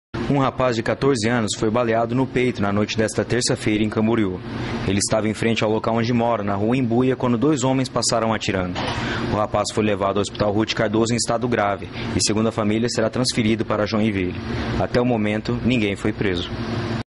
NO PORTÃO DE CASA: Menino de 14 anos foi atender amiga e levou tirombaço
Dois pistoleiros ainda crivaram de balas a casa do garoto